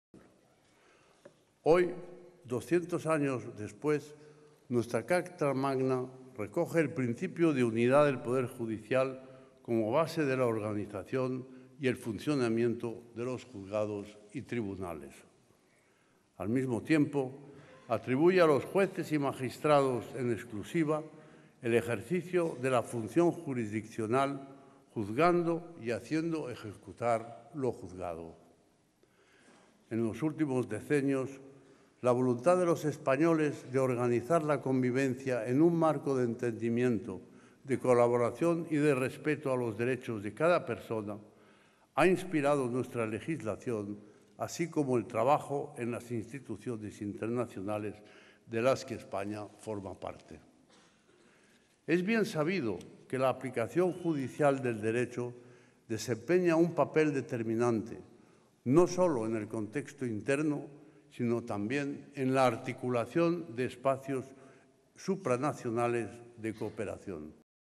Rey: "La Constitución atribuye justicia"
El Rey Juan Carlos ha recordado que la Constitución otorga a los jueces y magistrados "en exclusiva" la atribución de juzgar y ejecutar lo juzgado, durante su discurso en el acto de entrega de los despachos a la LXII Promoción de Carrera Judicial.